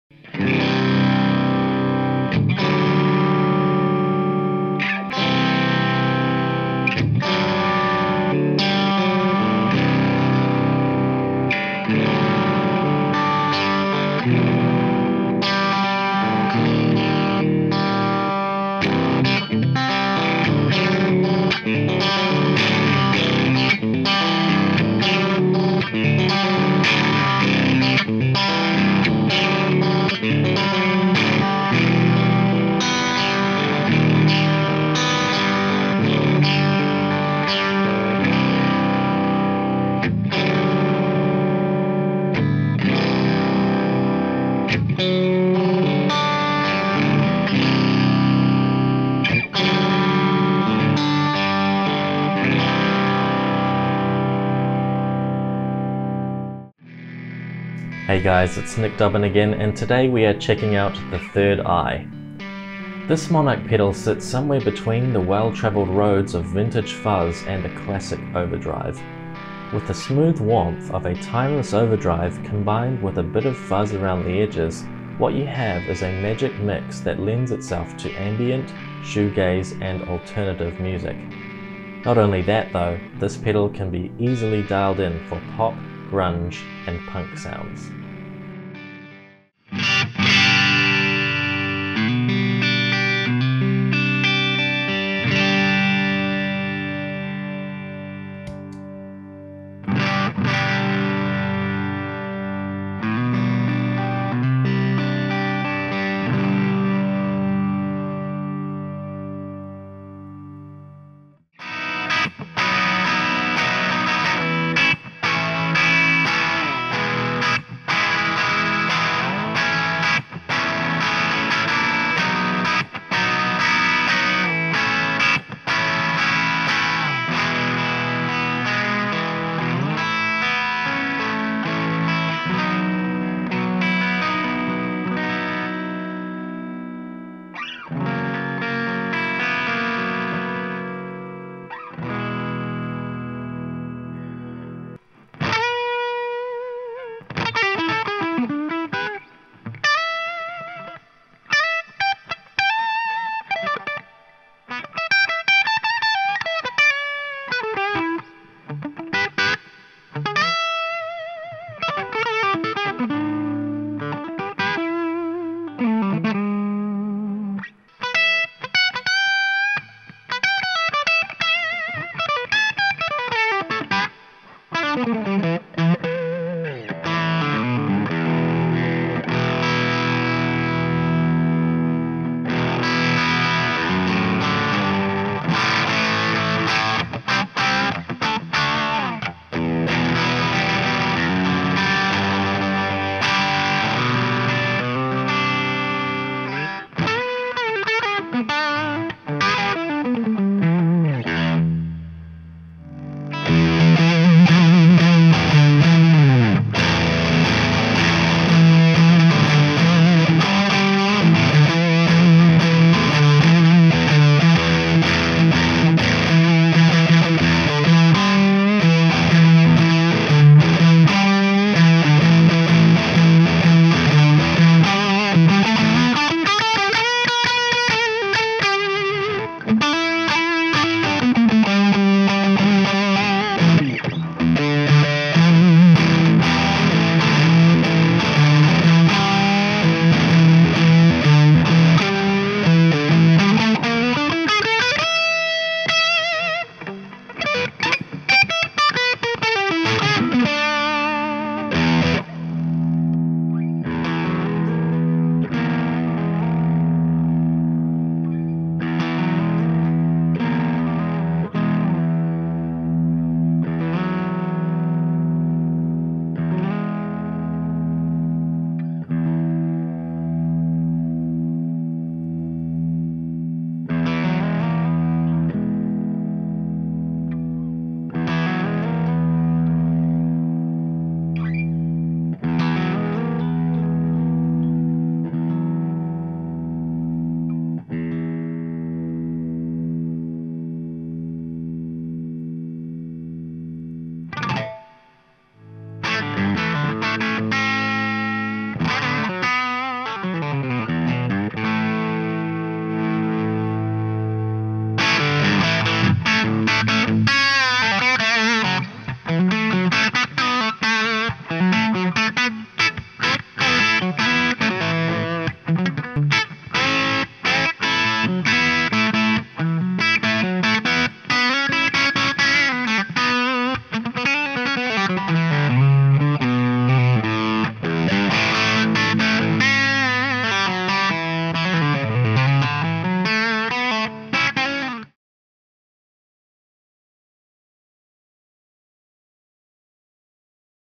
Monarch Pedals The Third Eye Overdrive Pedal Demo
The Third Eye is a dirt pedal that takes a timeless overdrive sound and puts some fuzz around the edges. This combination untaps some great shoegaze potential as well as all the usual pop and classic rock sounds.

1:29 - Demo begins.